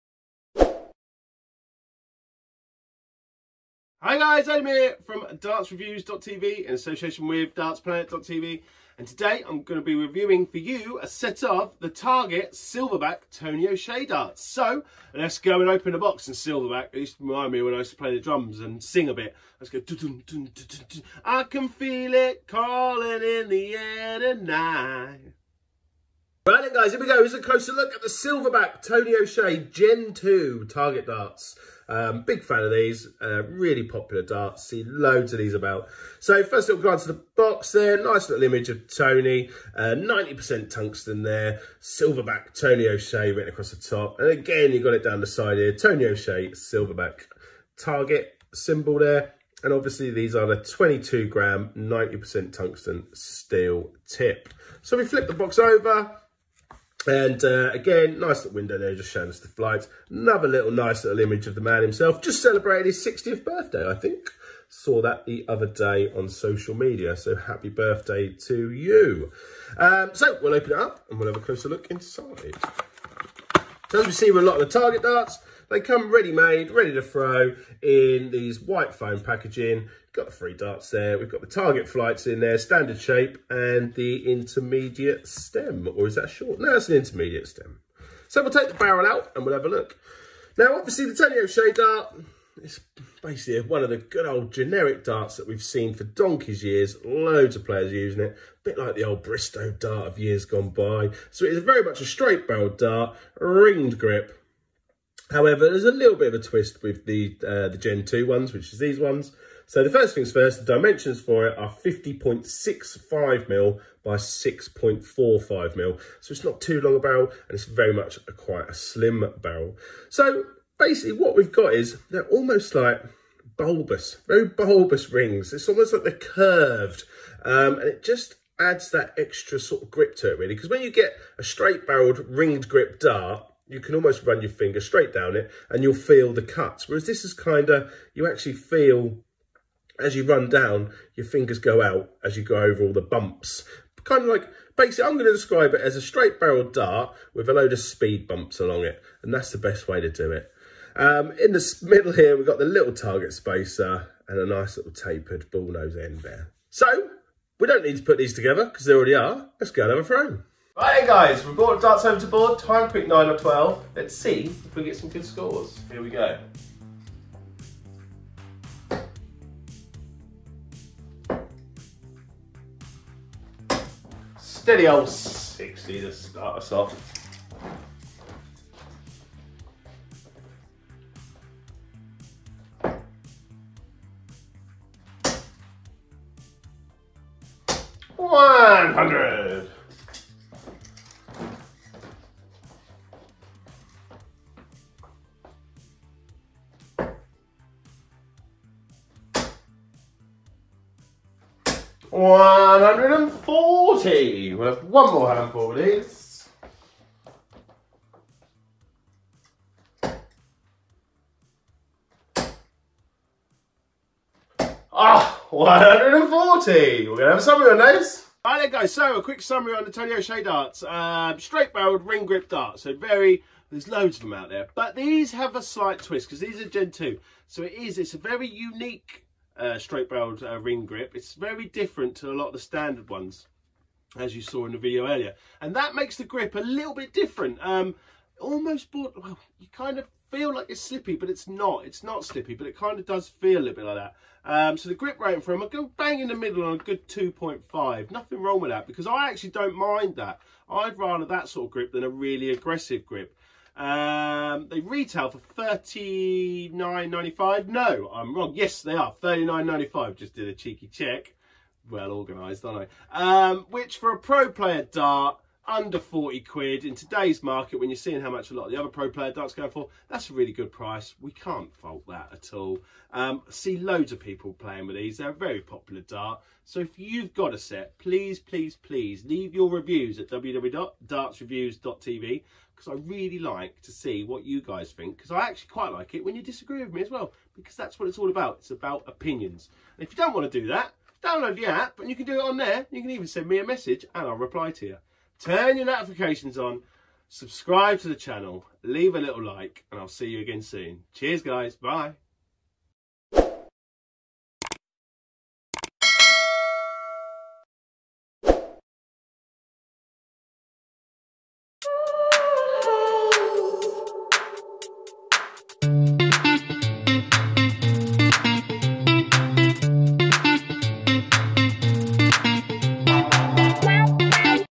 TONY O SHEA SILVERBACK GEN 2 DARTS REVIEW WITH ADAM WHITE
Adam gives his honest review of the Tony O’Shea Gen 2 Target Darts

Tony O’Shea Gen 2 Darts Description

As used by BDO England Captain Tony 'The Silverback' O'Shea.

This 90% Tungsten dart is beautifully contoured and features a classic grip form to produce a dart with a superb grip and feel.

This dart comes complete with Target Pro Grip Shafts, Target Vision Flights and bears Target's Lifetime Barrel Guarantee.

3 x Dart Barrels
3 x Pro Grip Shaft Black
3 x Tony O'Shea Flights
3 x 26MM Nickel Point

WEIGHT (E.G. 21)     BARREL LENGTH (E.G. 50) DIAMETER
22g                             50.65mm                                 6.45mm
24g                             50.65mm                                 6.7mm
26g                             50.65mm                                 7mm